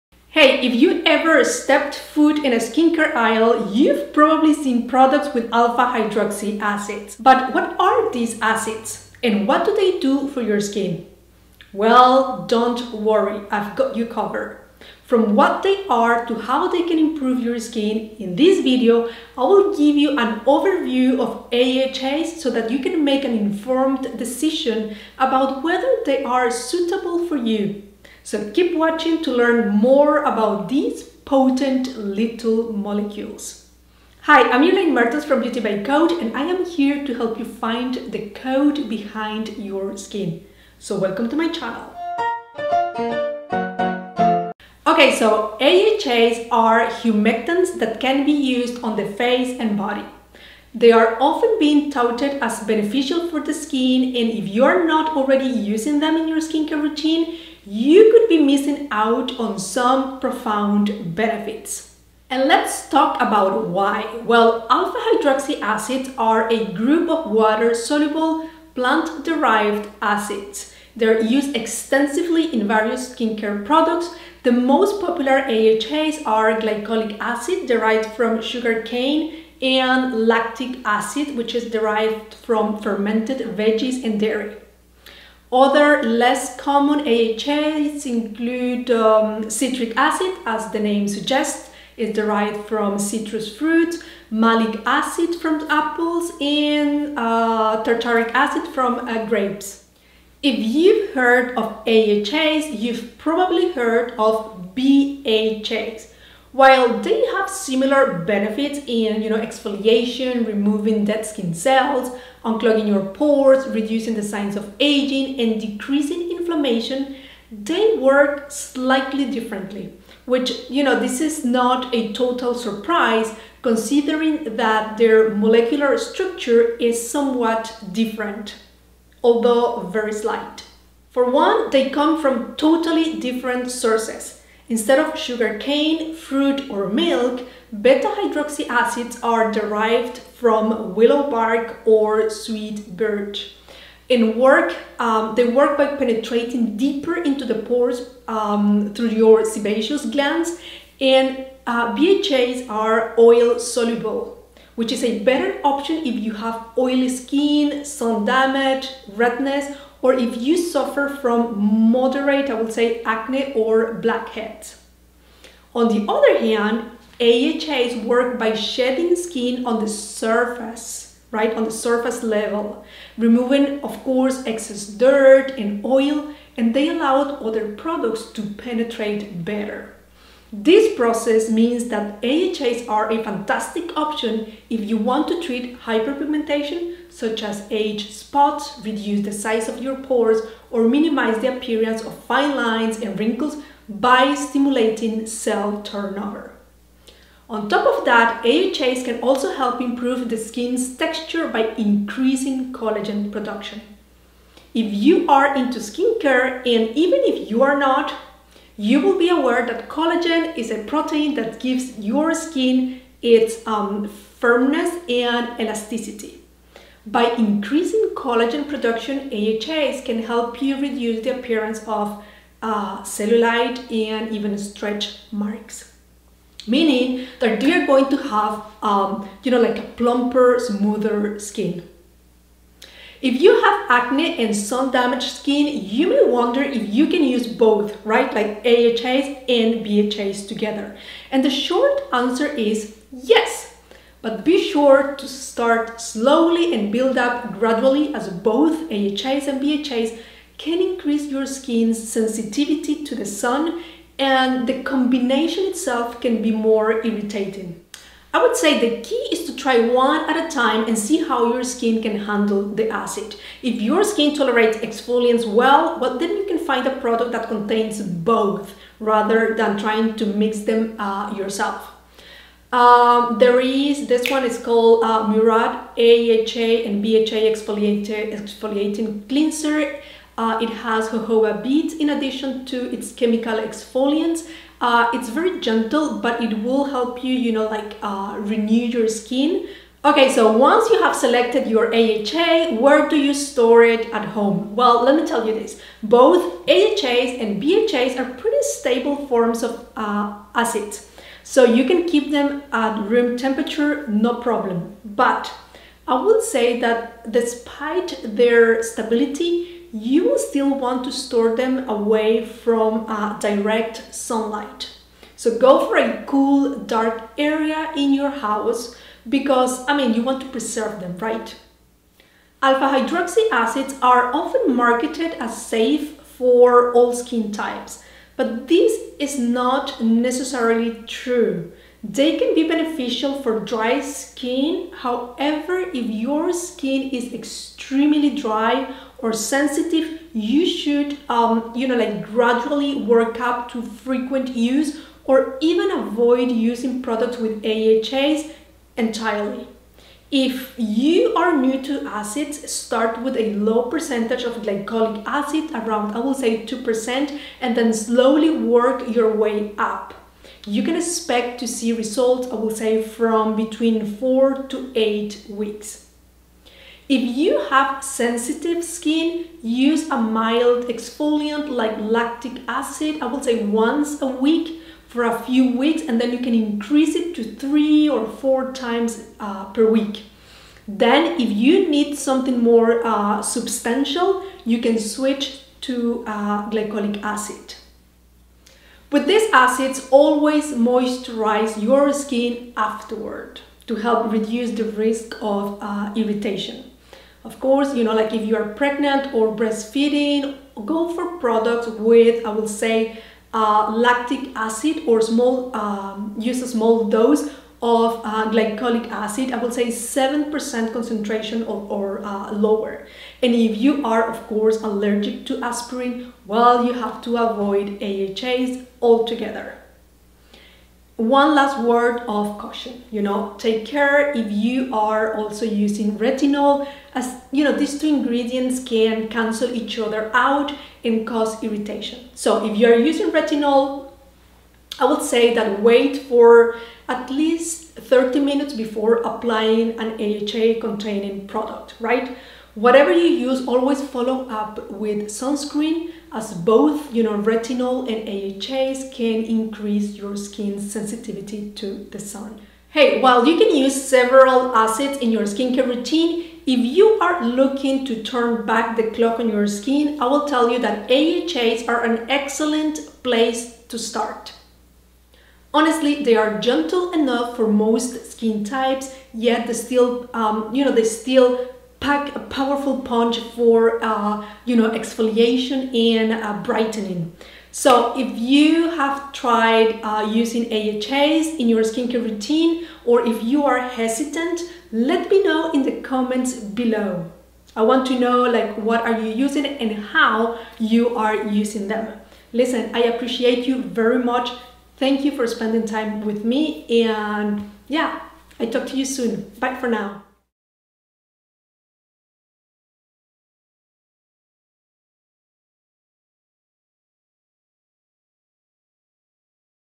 Alpha Hydroxy acids in skincare | acids 101
Are you wondering what are Alpha Hydroxy Acids and how you can include in your skincare routine? Well, this video is an Introduction To AHAs In Skincare! You’ll learn what are these acids, and what do they do for your skin. I’ll show you how they can improve your skin. I’ll give you an overview of AHAs so that you can make an informed decision about whether they’re suitable for you - so keep watching to learn more about these potent little molecules.

QUIZ
Want flawless skin? Acids are the answer!
Take the quiz and find out what acid is best for you.